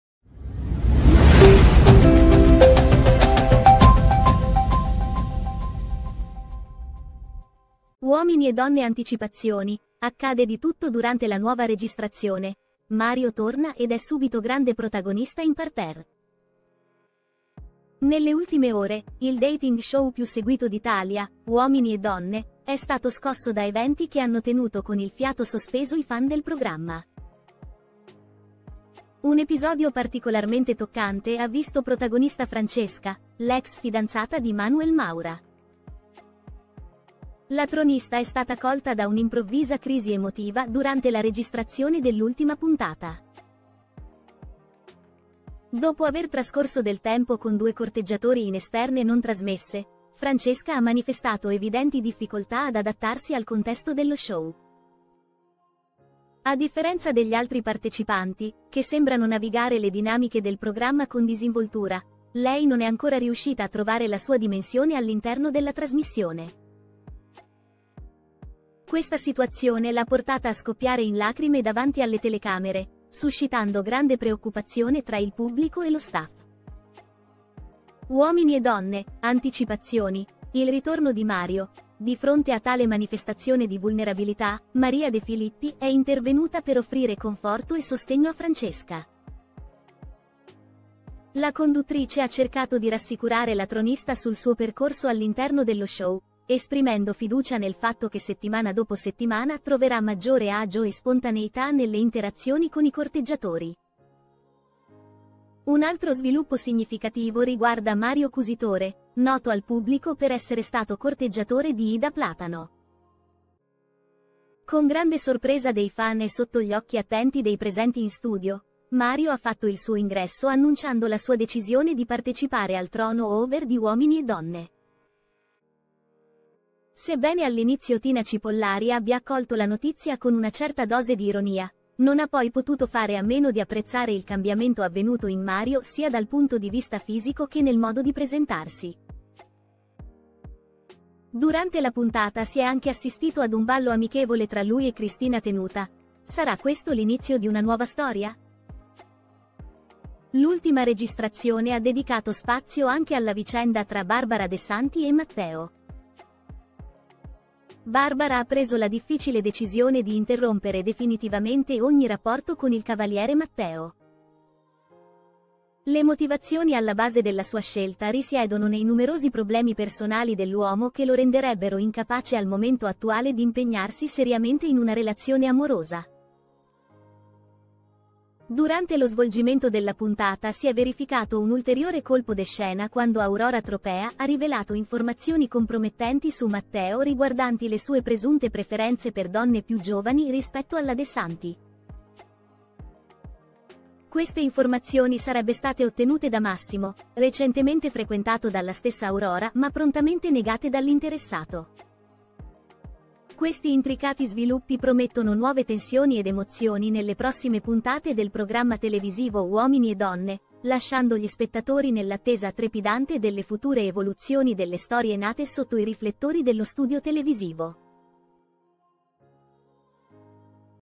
Uomini e Donne Mario ha deciso, subito protagonista in studio “Torna per un motivo preciso”
Uomini e Donne Mario ha deciso, subito pro
Uomini e Donne anticipazioni, accade di tutto durante la nuova registrazione: Mario torna ed è subito grande protagonista in parterre.

Nelle ultime ore, il dating show più seguito d’Italia, Uomini e Donne, è stato scosso da eventi che hanno tenuto con il fiato sospeso i fan del programma. Un episodio particolarmente toccante ha visto protagonista Francesca, l’ex fidanzata di Manuel Maura. La tronista è stata colta da un’improvvisa crisi emotiva durante la registrazione dell’ultima puntata.

tagonista in studio “Torna per un motivo preciso”